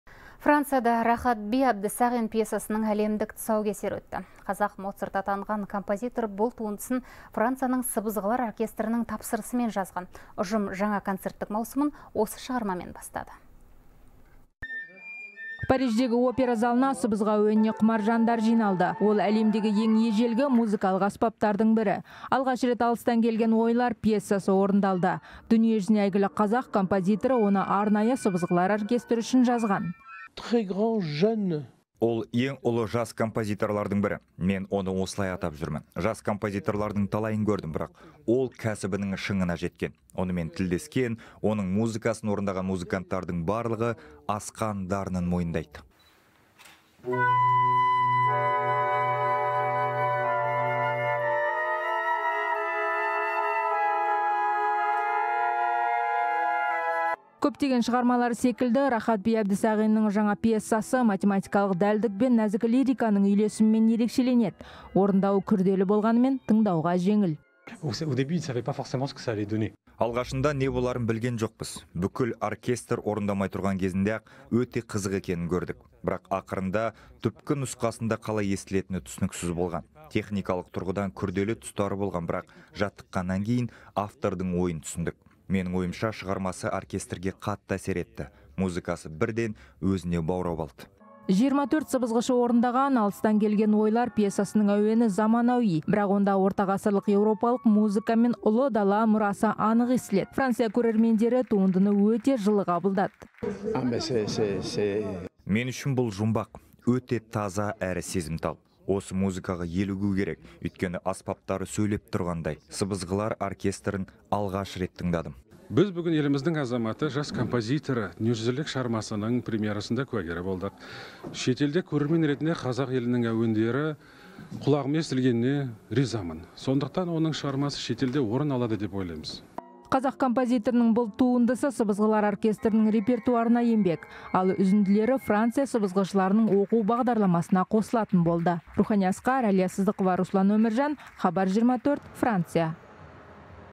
Францияда «Қазақ моцарты» пьесасының тұсаукесері өтті
Францияда Рахат би Әбдісағин пьесасының әлемдік тұсаукесері өтті. «Қазақ Моцарты» атанған композитор бұл туындысын Францияның сыбызғылар оркестрінің тапсырысымен жазған, деп хабарлайды «Хабар 24».